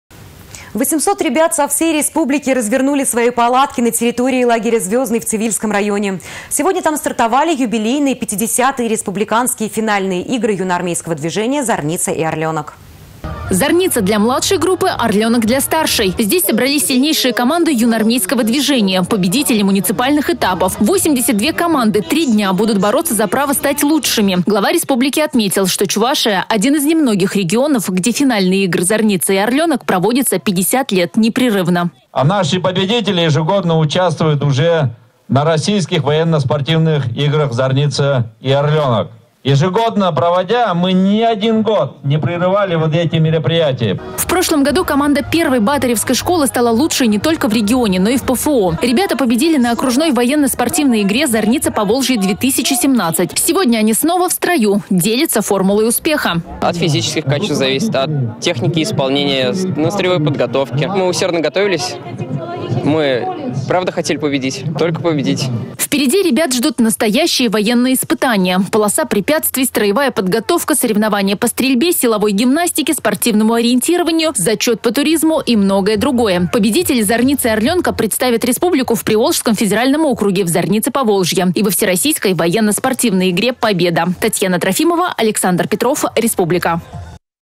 800 ребят со всей республики развернули свои палатки на территории лагеря Звездный
В Цивильском районе стартовали юбилейные пятидесятые республиканские финальные игры юнармейского движения «Зарница» и «Орленок».